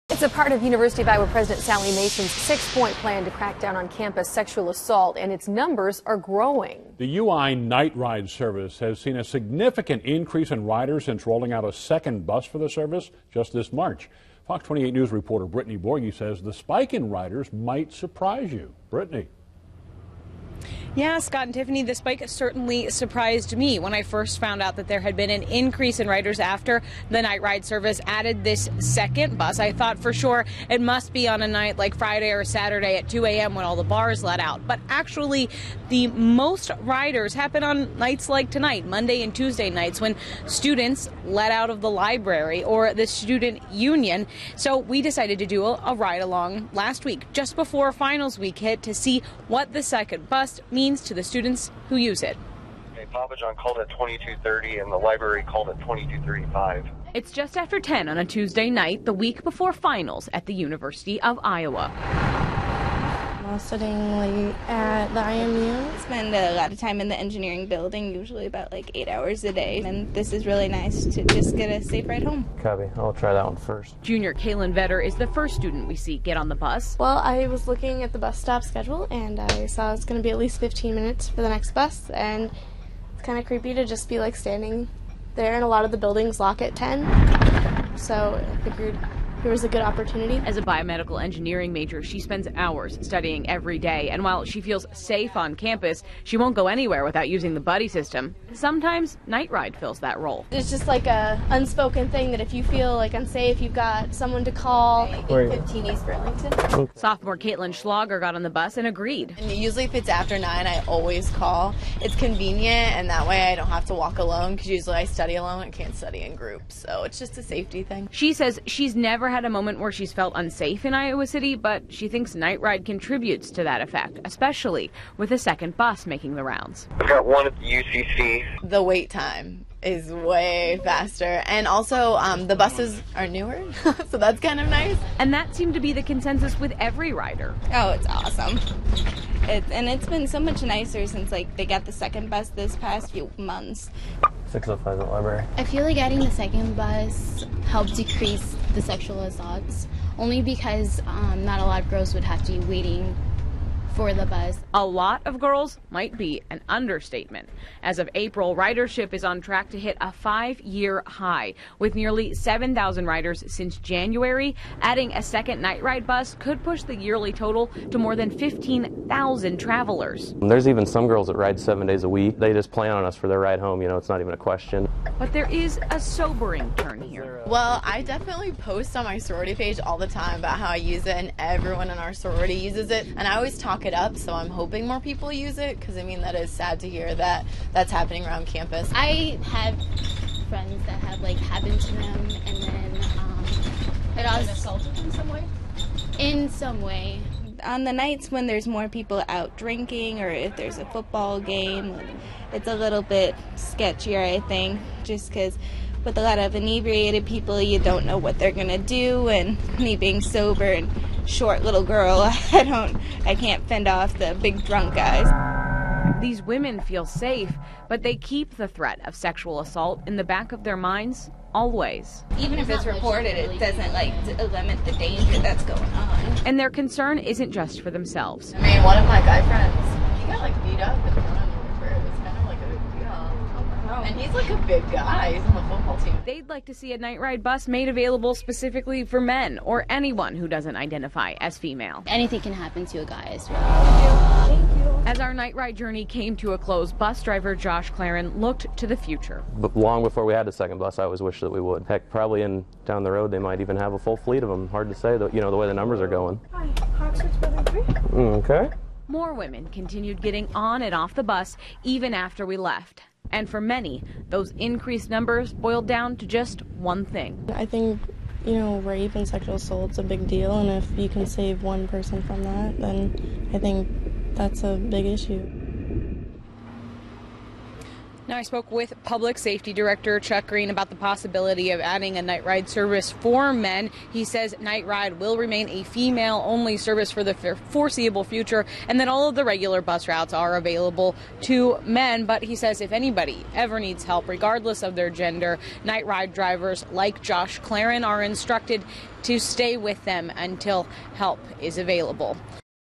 SPECIAL REPORT: UI Nite Ride
IOWA CITY, IA (CBS2/FOX28) -  It's part of University of Iowa President Sally Mason's six-point plan to crack down on campus sexual assault, and it's numbers are growing.

The UI "Nite Ride" Service has seen a significant increase in riders since rolling out a second bus for the service, just this March.